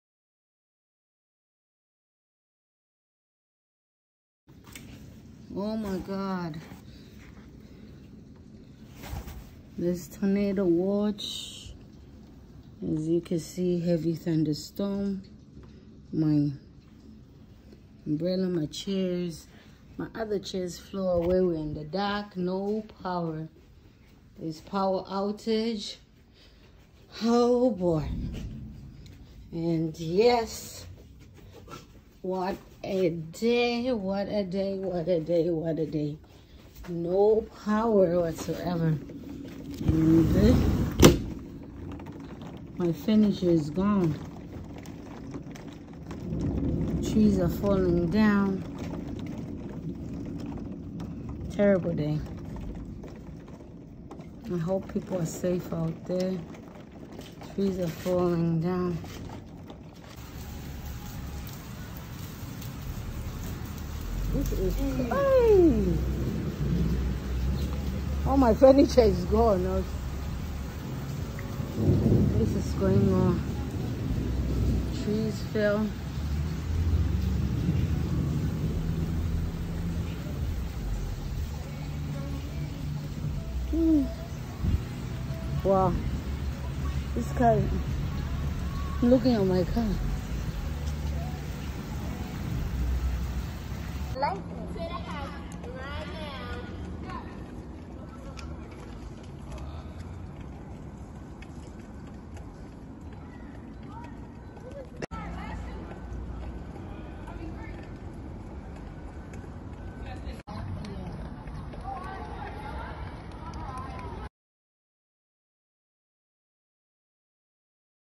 That was a tornado watch and heavy storm Hit us yesterday we still don’t have power hot hot prayers
Not a easy thing to to be in the heat with no air condition and no electricity prayer So they can resolve this quick🥲